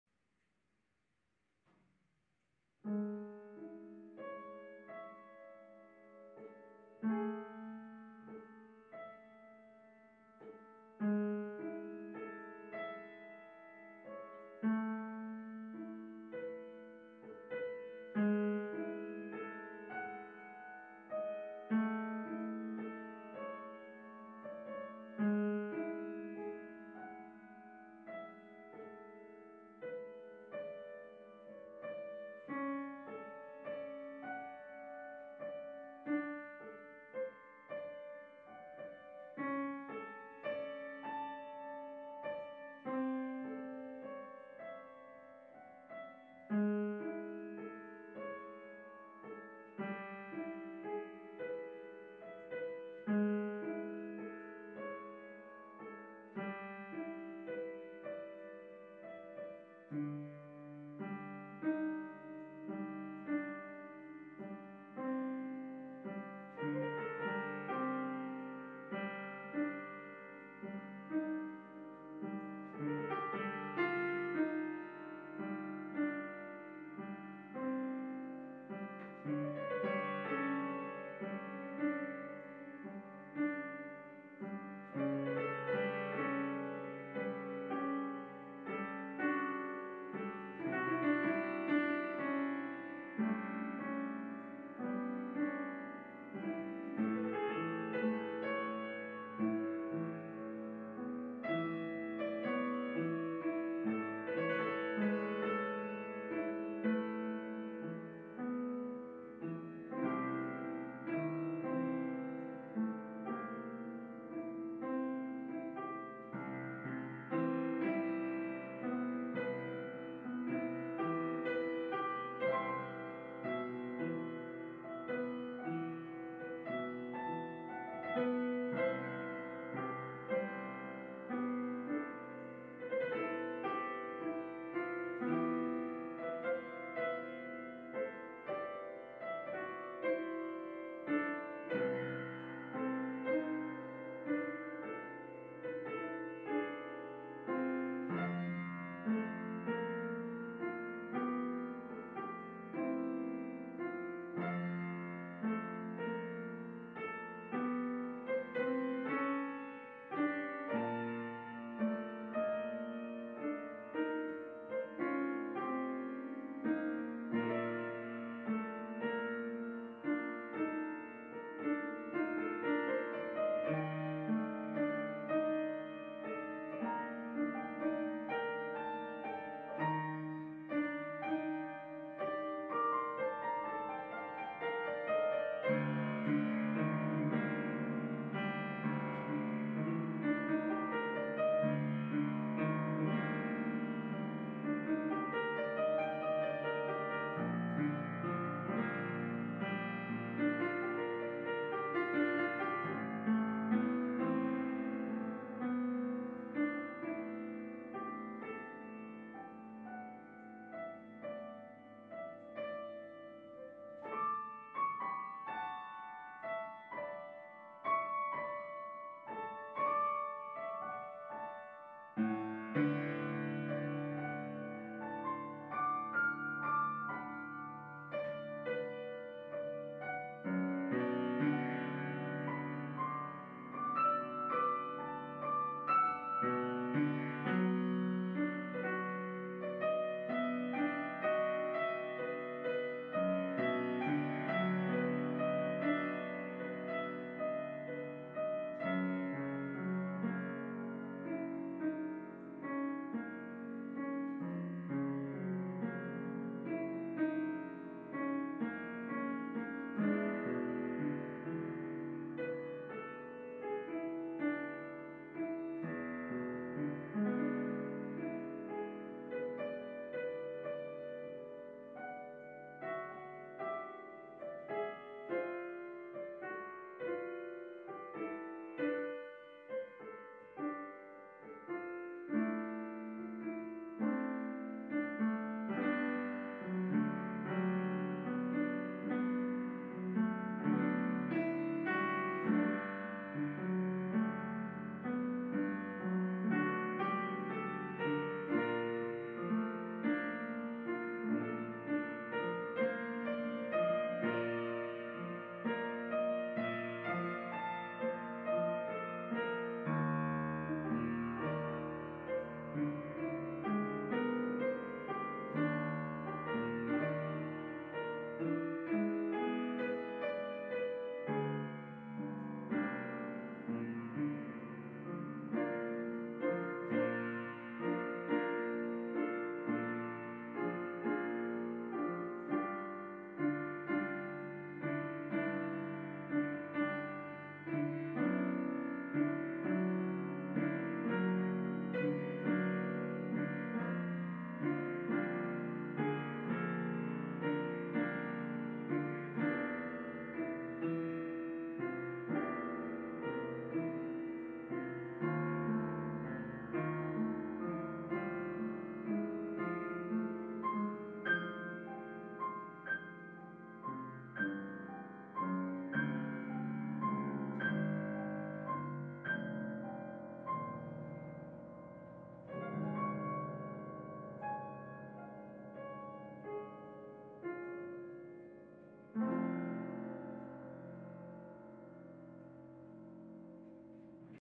Piano Improvisation: Berceuse (Like a Lullaby)
Was in a feeling for a "Wiegenlied". So I improvised this spontaniously on the piano. Mixed consonant and dissonant chords. The rhythm is rocking softly in a berceuse like rhythm, intending a lullaby mood.